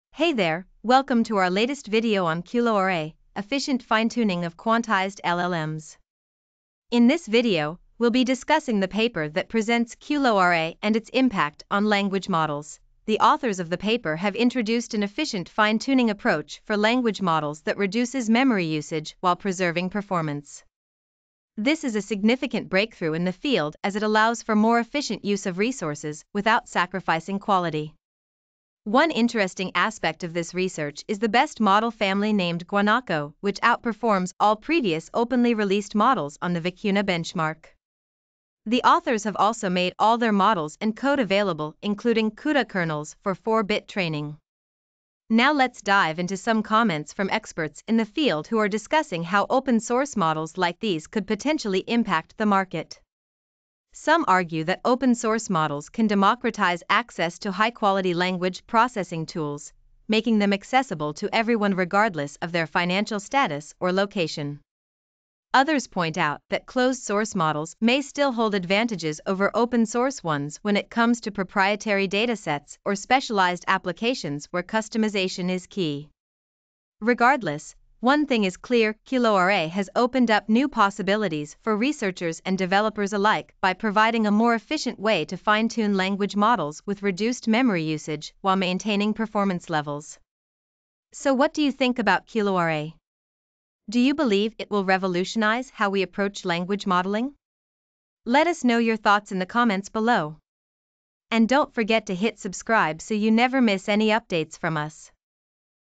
Efficient QLoRA Language Model Finetuning for Improved Performance and Reduced Memory Usage.
The paper presents QLoRA, an efficient finetuning approach for language models that reduces memory usage while preserving performance. The best model family, named Guanaco, outperforms all previous openly released models on the Vicuna benchmark. The authors release all of their models and code, including CUDA kernels for 4-bit training. The comments discuss the potential impact of open source models on the market and the advantages and disadvantages of closed and open source models.